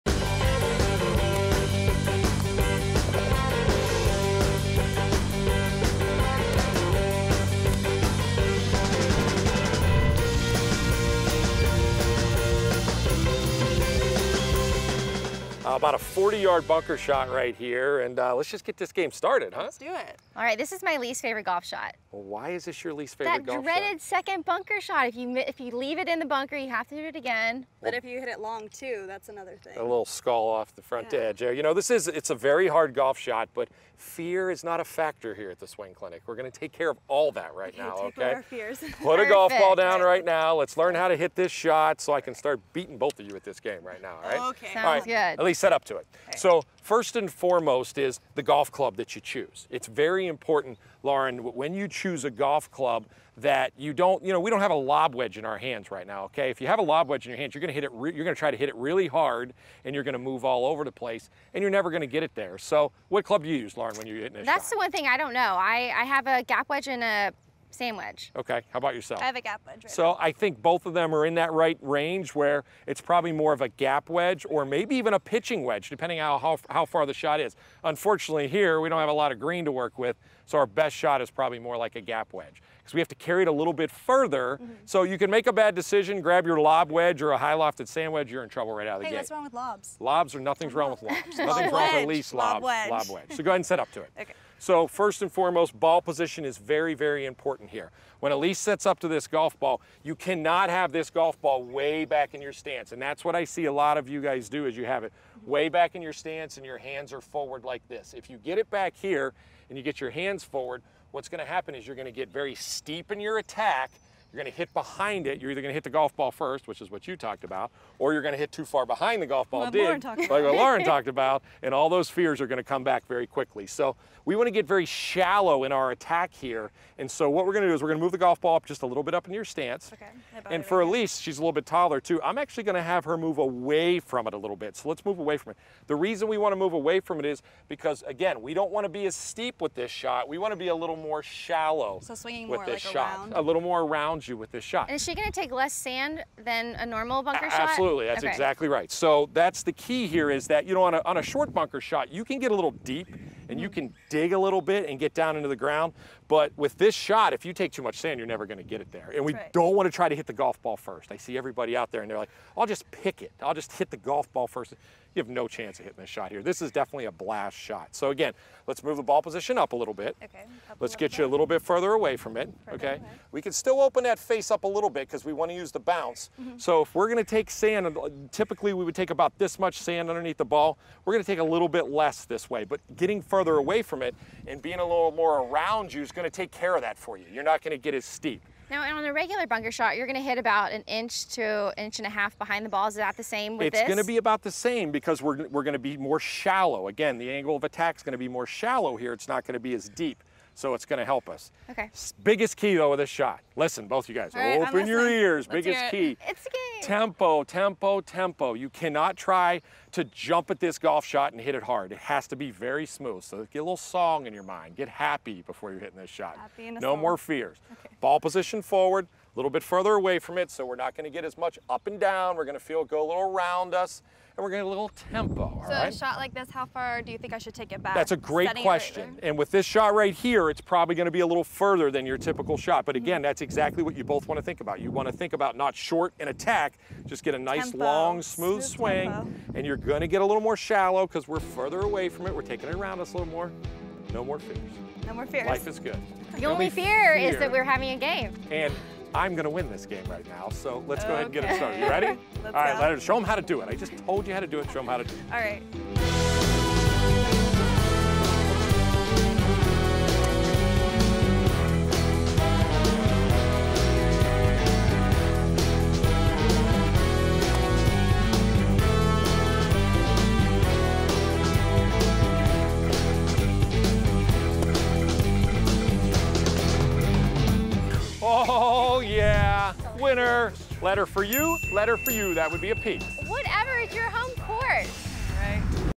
Swing Clinic: Jimmy, Lauren & Elise show you the 40-yard bunker shot!
Check out PGA Professional Jimmy Hanlin as he's joined by Elise Lobb and Lauren Olaya as they give you some Swing Clinic tips!